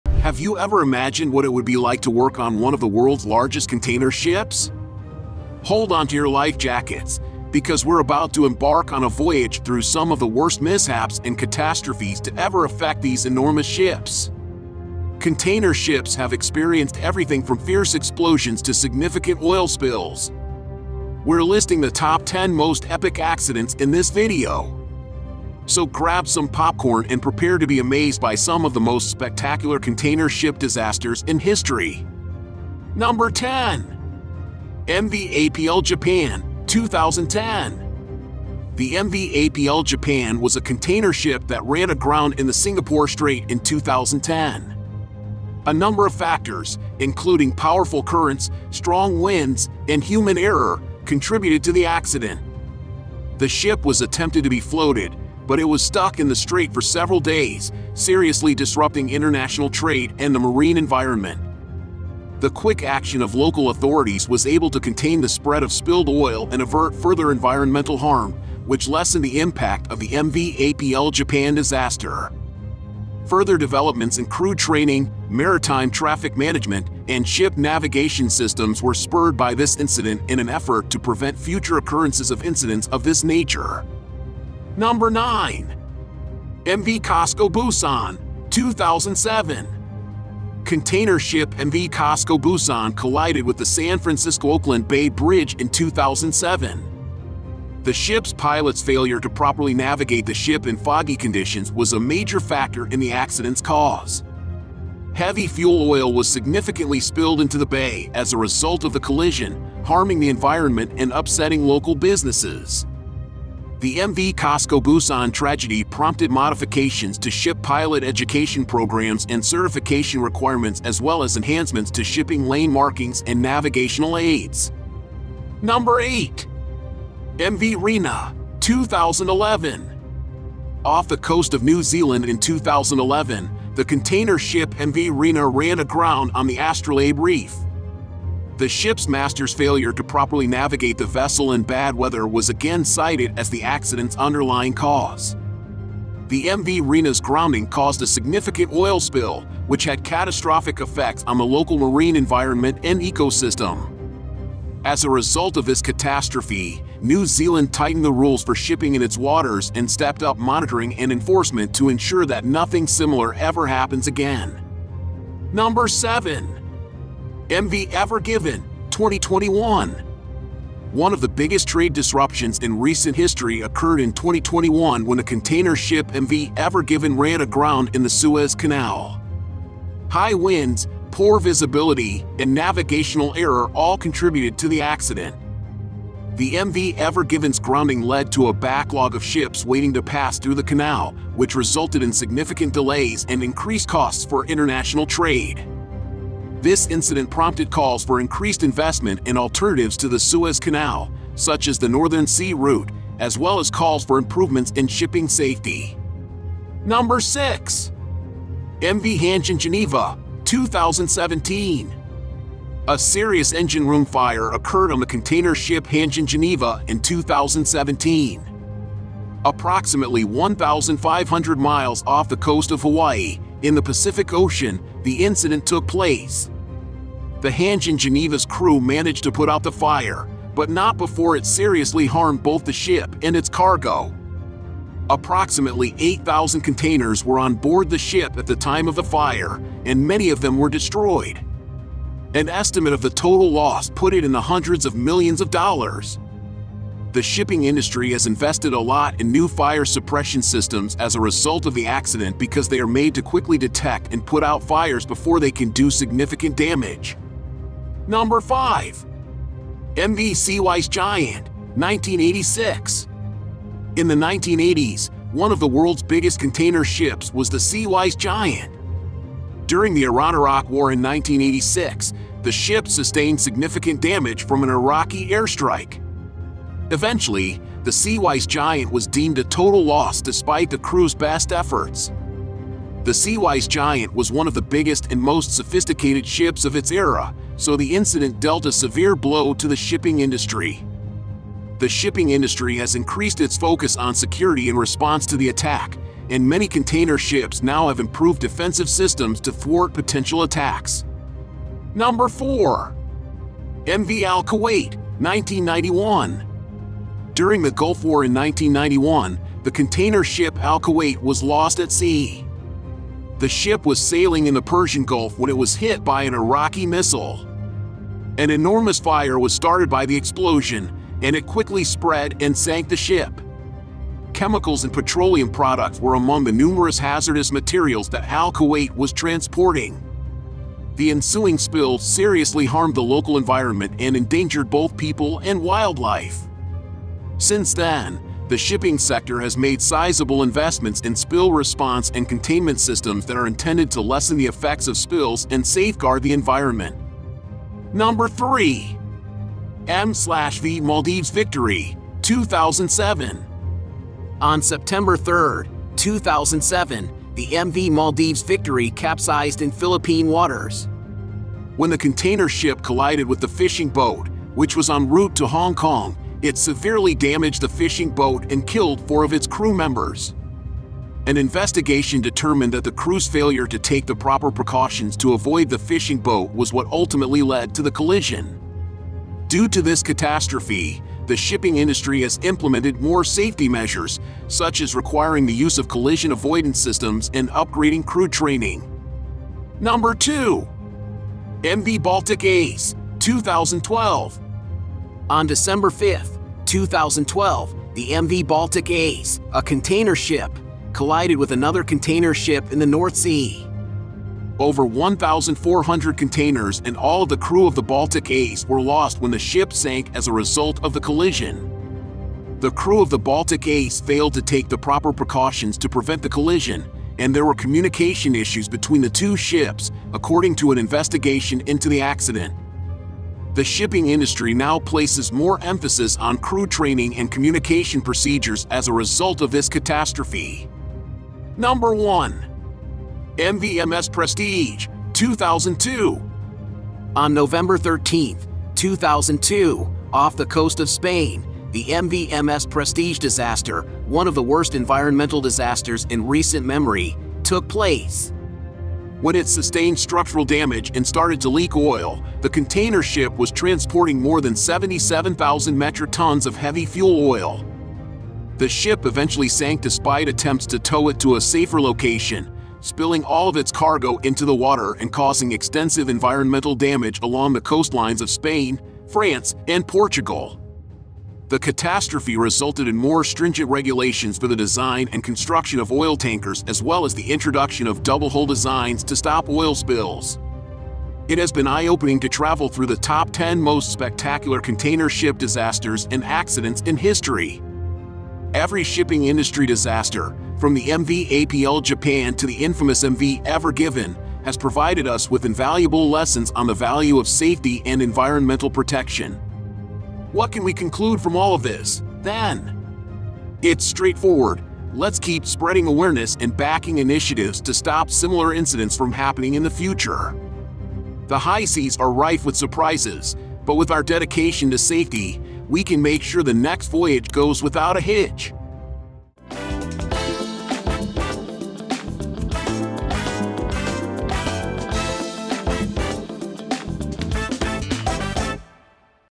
TOP 10 Most Shocking Container Ships Accidents and Disasters
Get ready to witness the most catastrophic container ship disasters in history! In this video, we've compiled the 𝗧𝗢𝗣 𝟭𝟬 𝗠𝗢𝗦𝗧 𝗦𝗛𝗢𝗖𝗞𝗜𝗡𝗚 𝗖𝗢𝗡𝗧𝗔𝗜𝗡𝗘𝗥 𝗦𝗛𝗜𝗣𝗦 𝗔𝗖𝗖𝗜𝗗𝗘𝗡𝗧𝗦 𝗔𝗡𝗗 𝗗𝗜𝗦𝗔𝗦𝗧𝗘𝗥𝗦, from massive explosions to terrifying collisions. These accidents and disasters show just how dangerous shipping can be. You won't believe your eyes as we take you on a journey through the most jaw-dropping footage of container ships in distress. Don't miss out on this unforgettable experience. Watch now and see why these container ships are among the most perilous vessels on the planet!

Play Rewind Repeat and Enjoy!!